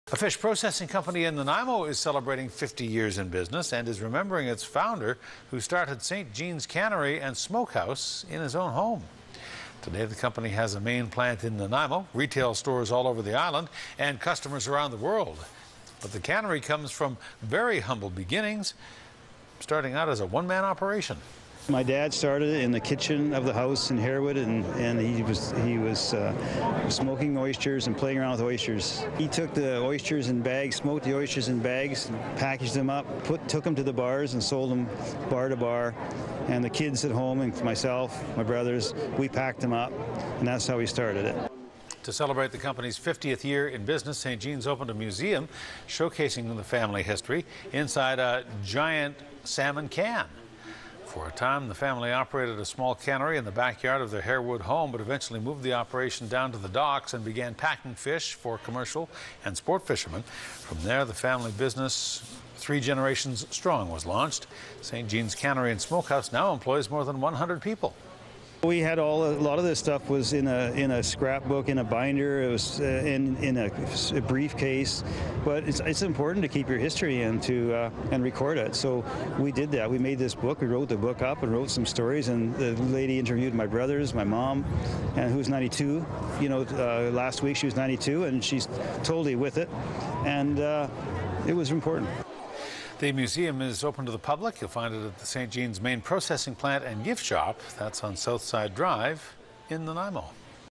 Saint Jean's Cannery and Smokehouse Celebrates 50 Years in Business
NANAIMO - A fish processing company in Nanaimo is celebrating 50 years in business and remembering its founder who started Saint Jean's Cannery and Smokehouse in his own home.

The company has a main plant in Nanaimo, retail stores all over the island and customers all over the world.  The company comes from very humble beginnings. It began as a one-man operation.

To celebrate the company's 50th year, Saint Jean's opened a museum showcasing the family history inside a giant salmon can.  For a time, the family operated a small cannery in the backyard of their Harewood home, but eventually they moved the operation down by the docks and began packing fish for commercial and sports fishermen

From there, a family business three generations strong launched. Saint Jean's Cannery and Smokehouse now employs more than 100 people.

The museum is open to the public at Saint Jean's main processing plant and gift shot on Southside drive in Nanaimo.